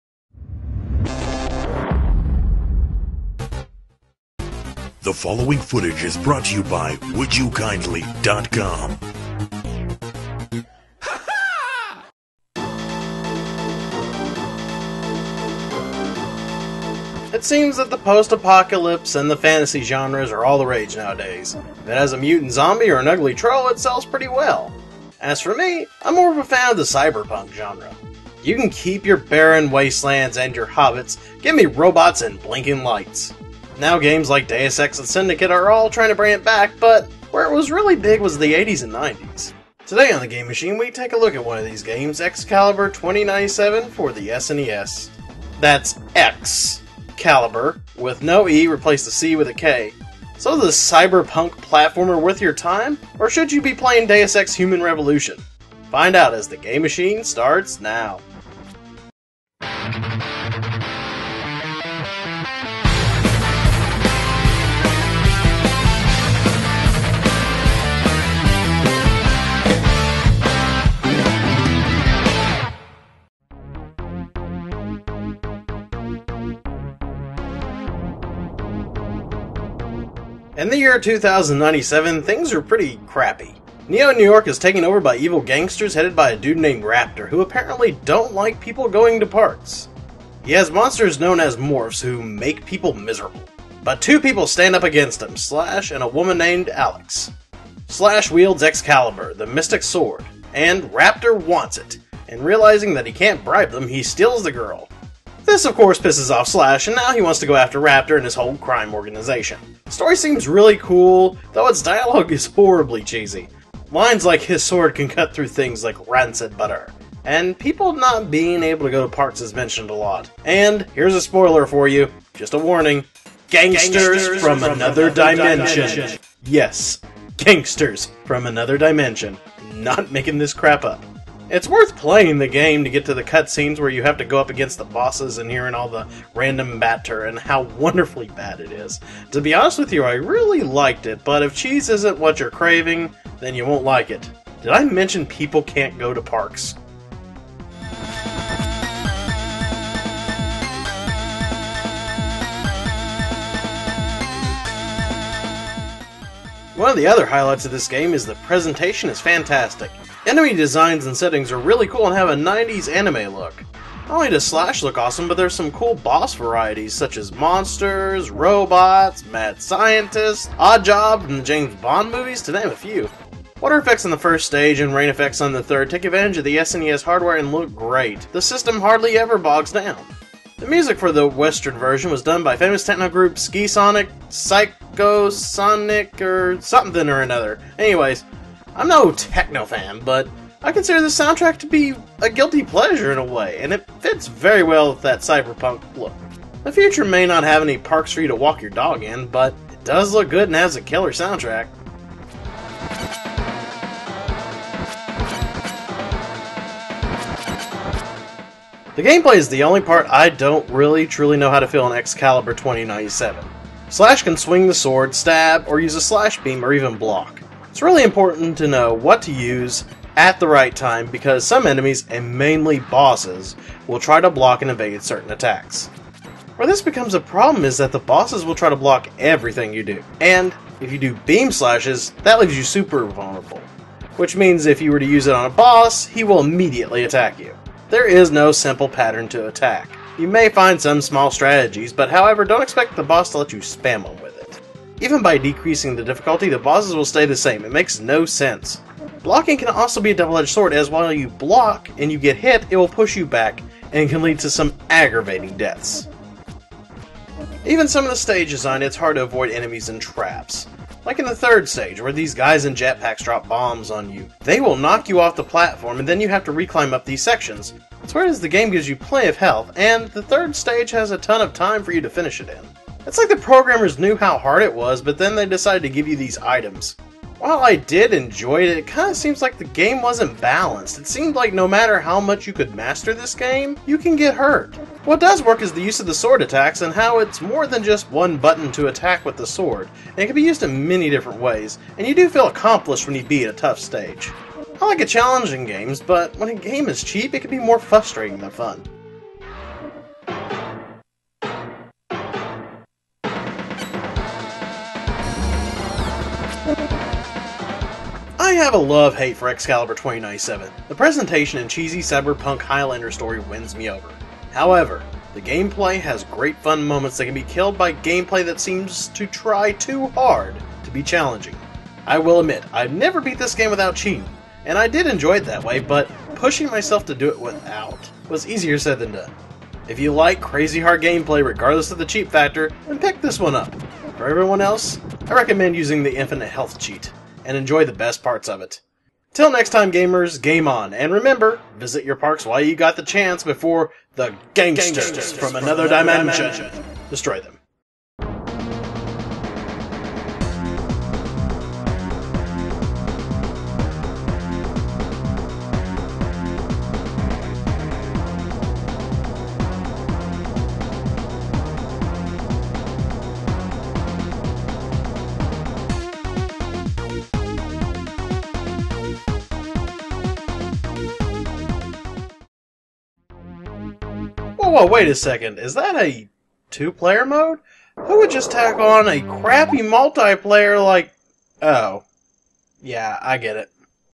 XKaliber 2097: The Game Machine SNES Review
What happens when you take Lord of the Rings and cross it with Blade Runner? You may get XKaliber 2097 for the SNES. Slash has a mystical sword XKaliber which he will use to take out Rapter's syndicate. Also his woman gets kidnapped bummer. Is this SNES game a hidden gem or should it be melted into scrap. Find out in this episode of The Game Machine.

for great gaming news, articles, podcast, and much more.